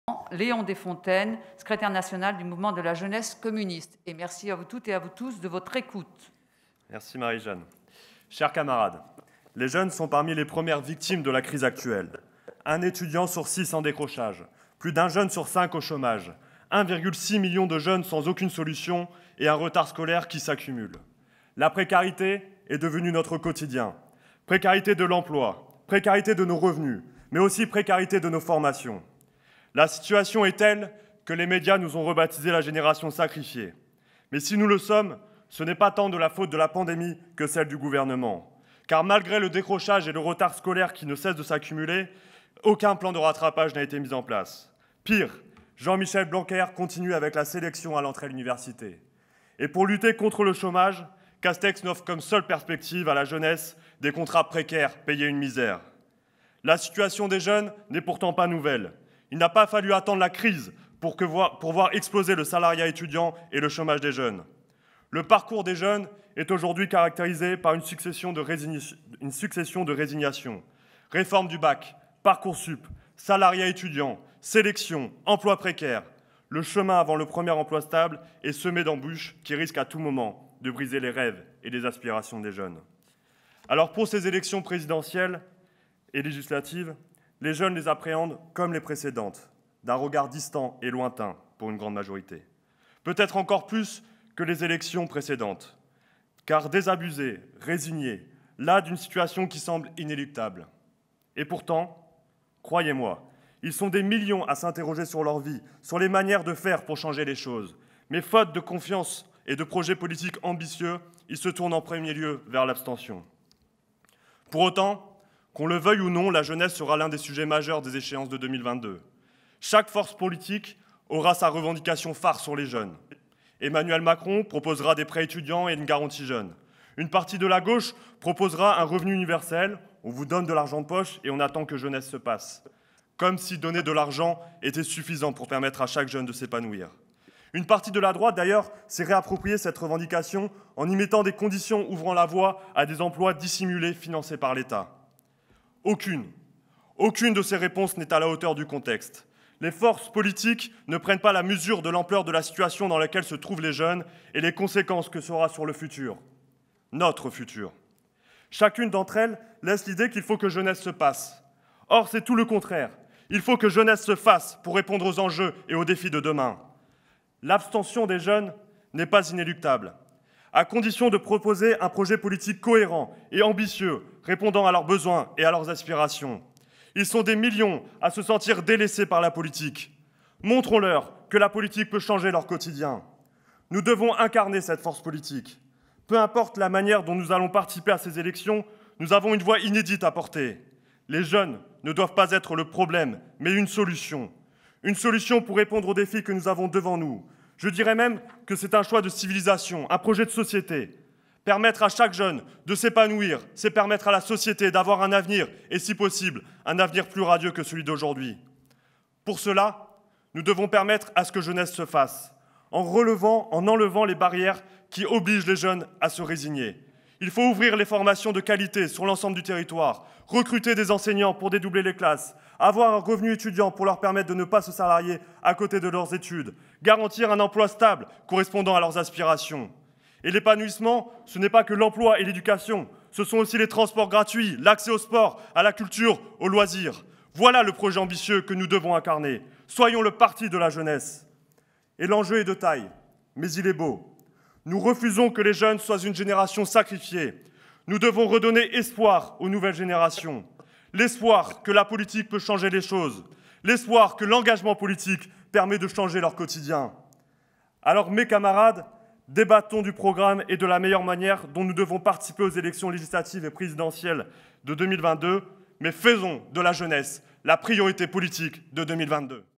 Faire de la jeunesse la priorité de 2022 Léon Deffontaines MJCF
Léon Deffontaines secrétaire général du MJCF